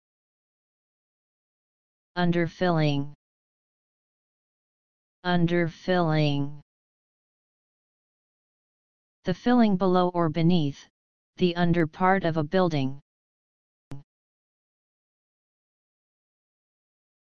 Underfilling Meaning
The filling below or beneath; the under part of a building.

In this video, you will learn "Underfilling" Meaning and pronunciation in English.

Wordy Affair